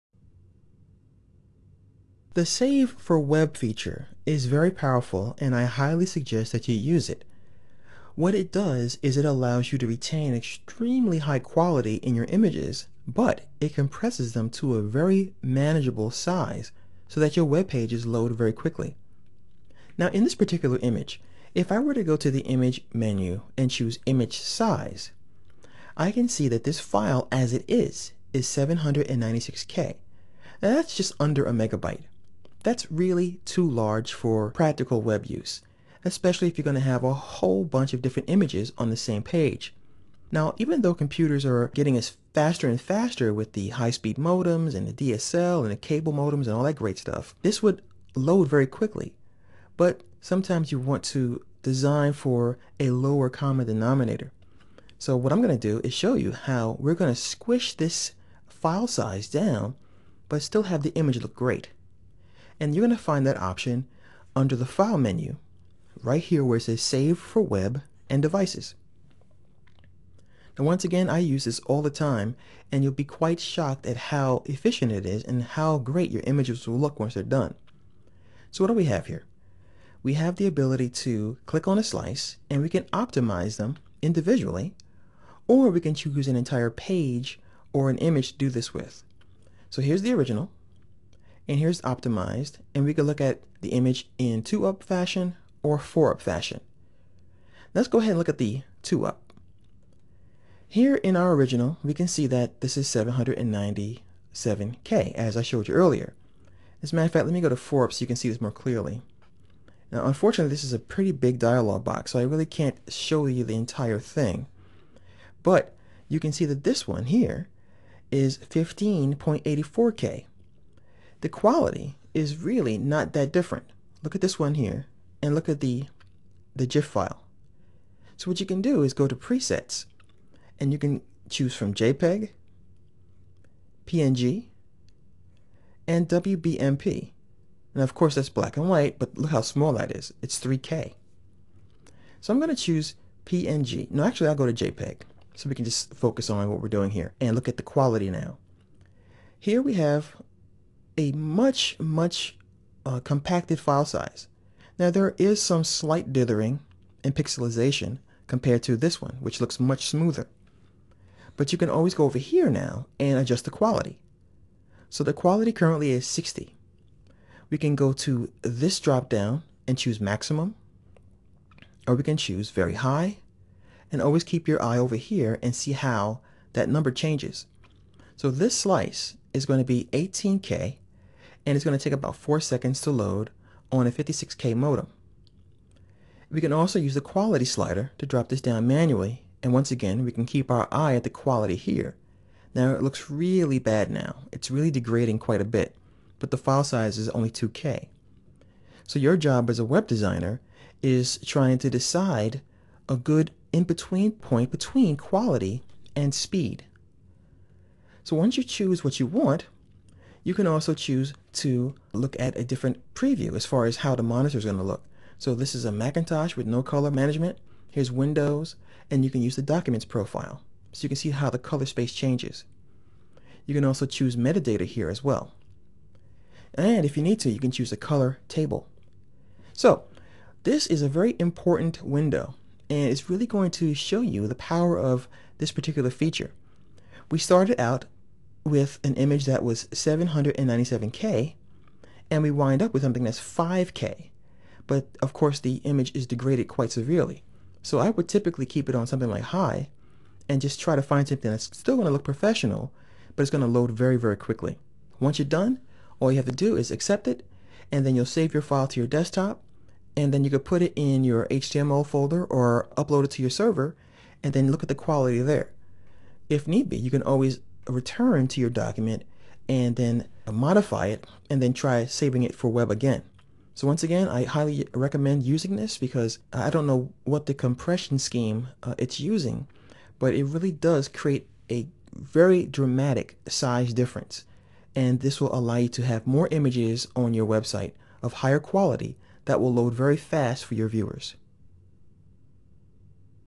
Photoshop Tutorials for Beginners - Slicing Images in Photoshop CS4
Visit this site to get tons of Photoshop tutorials like these delivered instantly to your computer!  This video was created by a friend of mine to show how to save images for the web using Photoshop CS4.